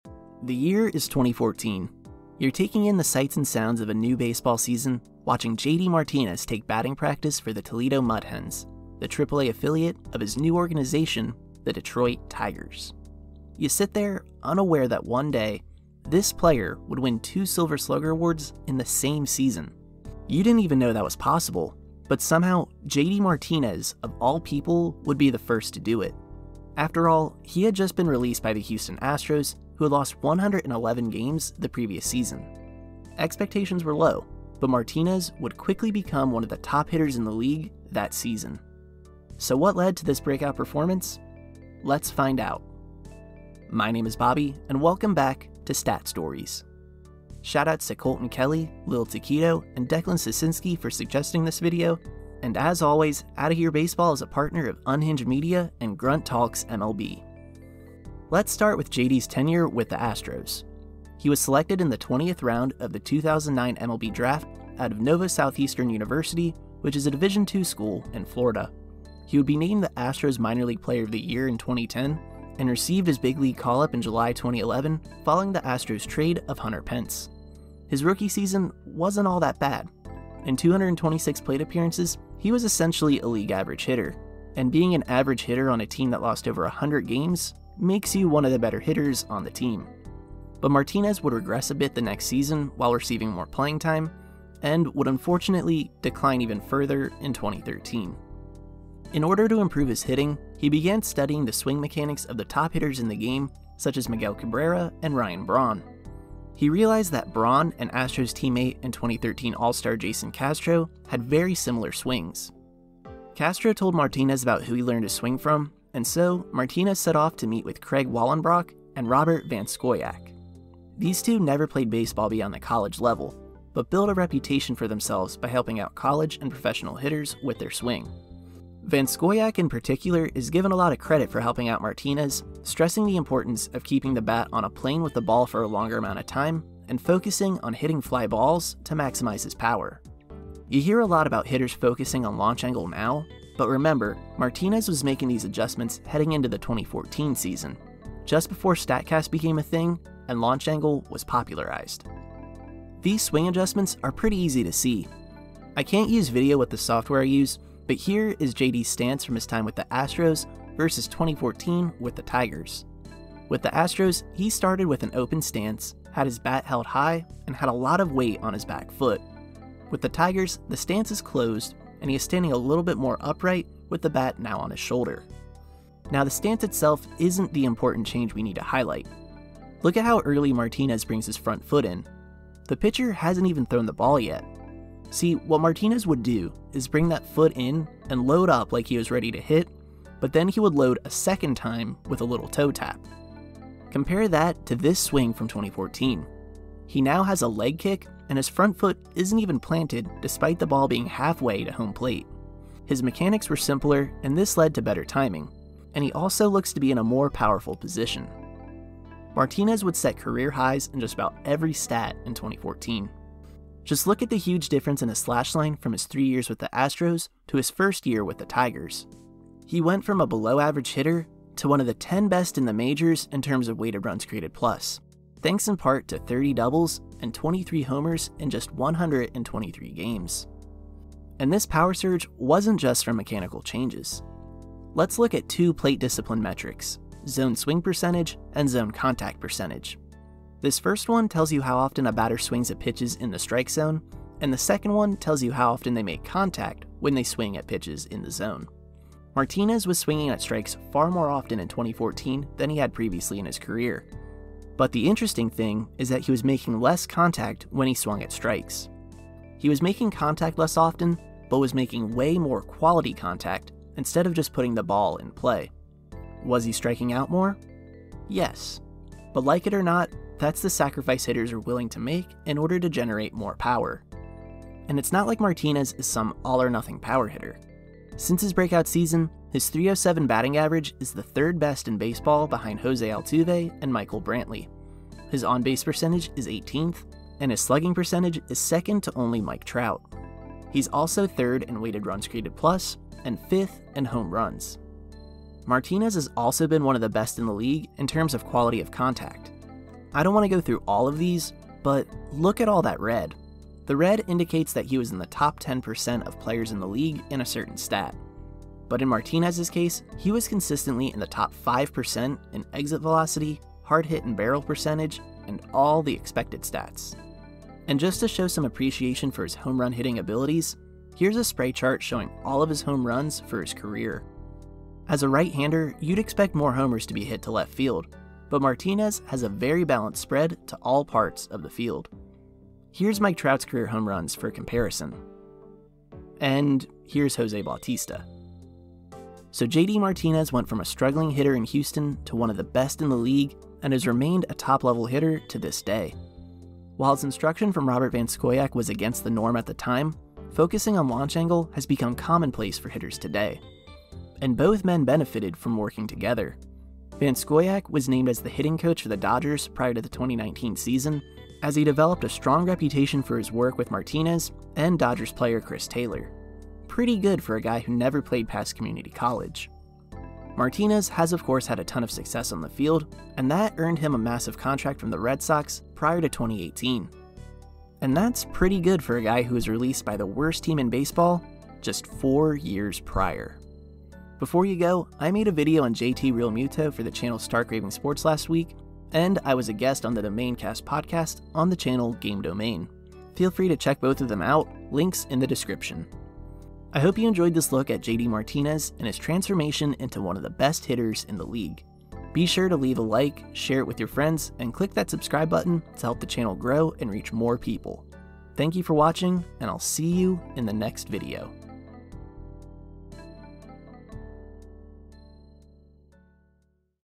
A Look at How JD Martinez Broke Out for the Detroit Tigers | Stat Stories
JD Martinez was released by the Houston Astros prior to the 2014 season. In other words, he was cut from a team that had lost 111 games the previous season. He would join the Detroit Tigers and became a surprise star for a division winning team. So what changed for him?

In this video I look at Martinez and his relationship with Craig Wallenbrock and Robert Van Scoyoc. These were two hitting instructors who had no professional baseball experience, but they revitalized the career of Martinez with instruction that was unorthodox at the time. Instead of focusing on line drives, Martinez would try to elevate the ball and maximize his power. This focus on launch angle has become a big part of hitting philosophies now, but wasn't popular at the time.

Martinez would swing and miss more often, but was still able to drive the ball to all parts of the field, and make more quality contact instead of just putting the ball in play. He remains a quality hitter to this day, and that earned him a massive contract from the Boston Red Sox in 2018. And Van Scoyoc benefited too, as he was named as hitting coach for the Los Angeles Dodgers prior to the 2019 season.

Shoutout to Lorenzo for the help with research!

Outta Here Baseball is a partner of Unhinged Media and Grunt Talks MLB